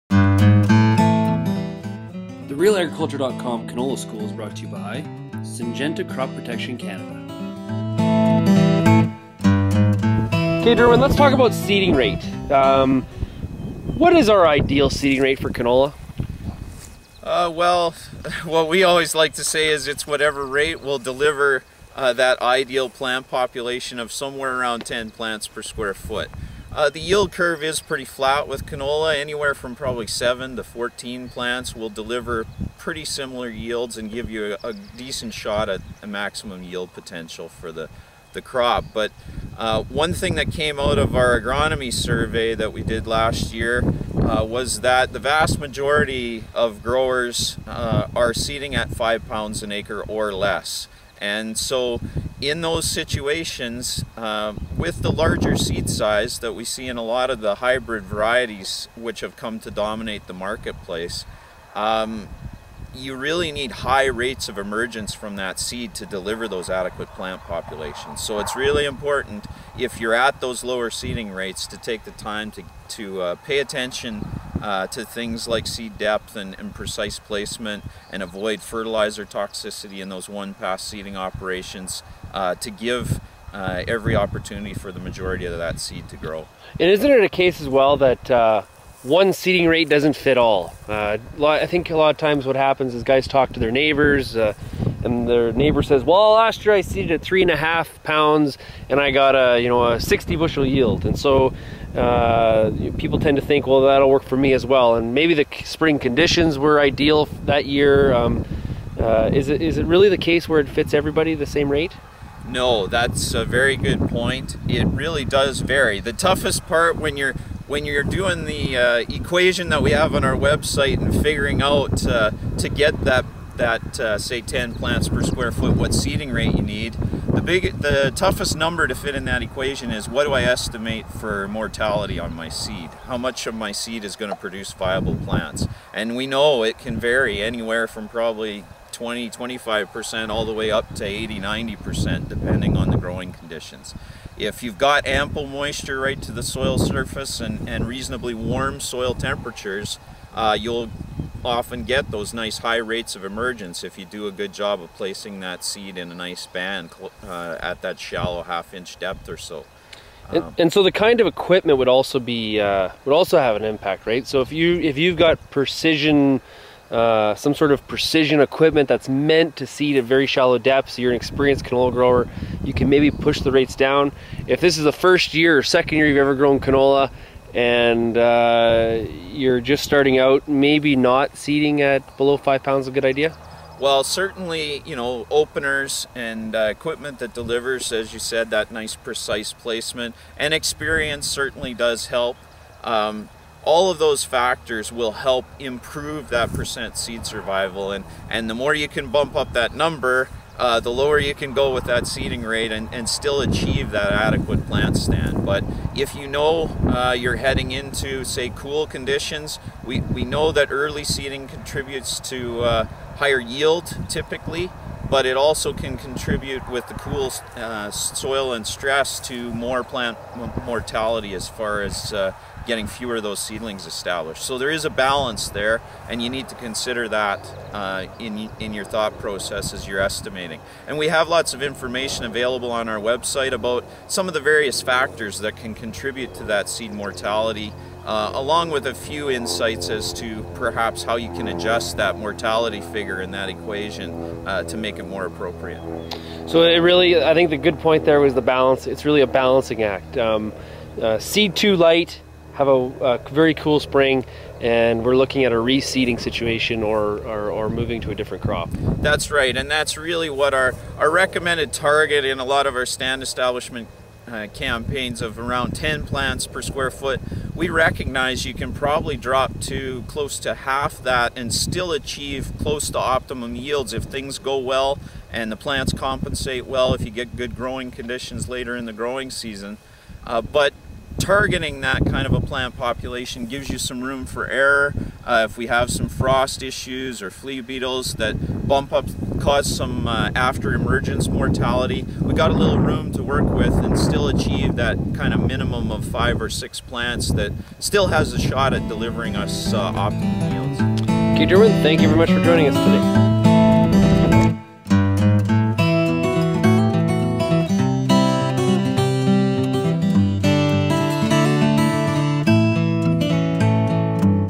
Canola School: What is the Ideal Seeding Rate?
and Derwyn Hammond from the Canola Council of Canada discuss the ideal seeding rate for canola to ensure success